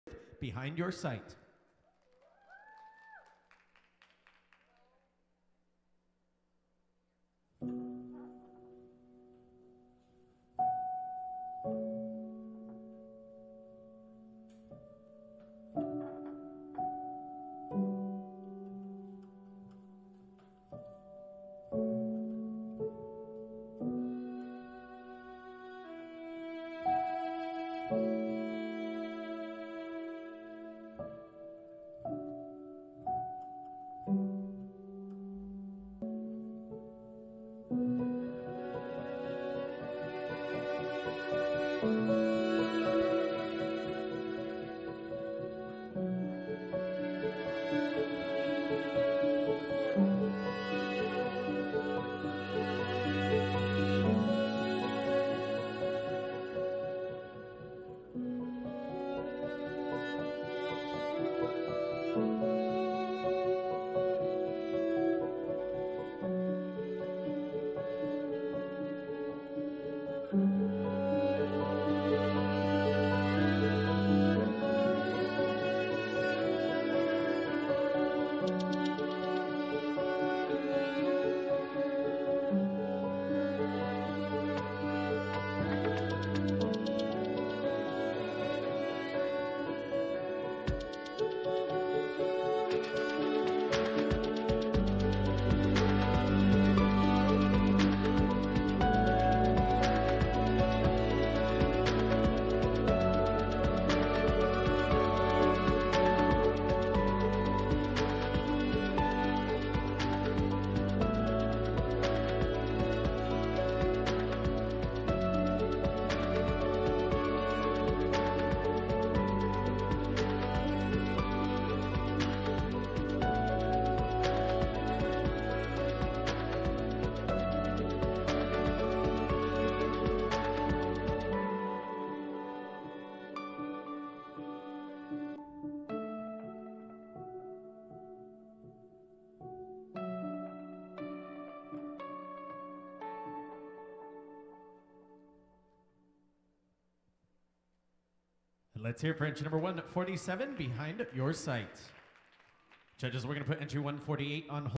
Kimberly Newman - Behind Your Sight
YYC Dance Project

8th Place Teen Solo

Radix Portland 2018